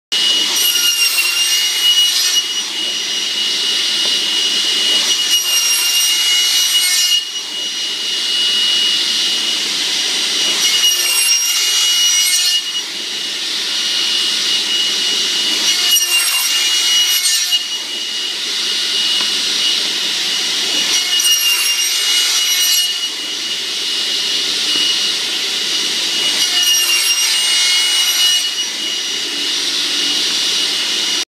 Cutting Dowel
The safe way to cut packs or hardwood dowel ramming.